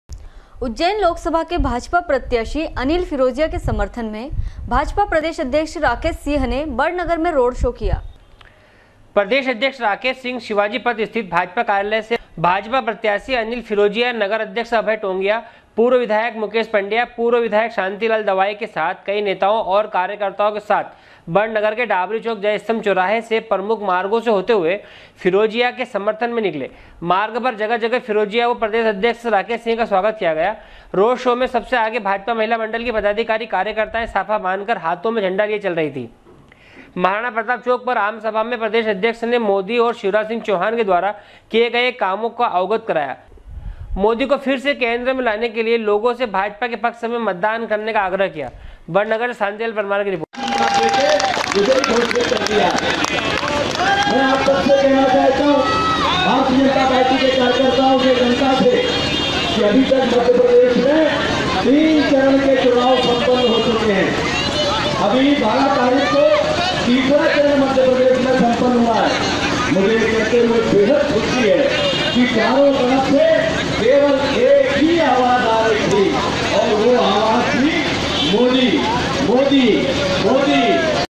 sw24news barnagar 16.05.2019
उज्जैन लोकसभा के भाजपा प्रत्याशी अनिल फिरोजिया के समर्थन में भाजपा प्रदेश अध्यक्ष राकेश सिंह ने बड़नगर में रोड शो किया